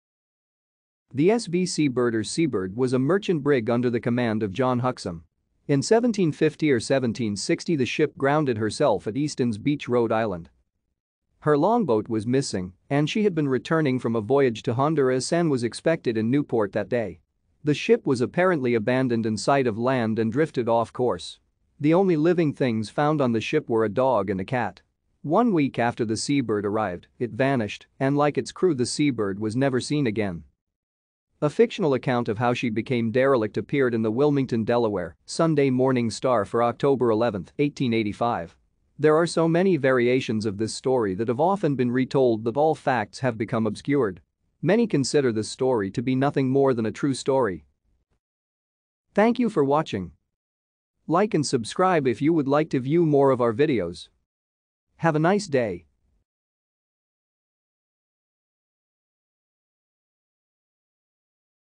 Sea Bird (Ship)
The SV Sea Bird or Seabird  was a merchant brig under the command of John Huxham. In 1750 or 1760 the ship grounded herself at Easton's Beach, Rhode Island.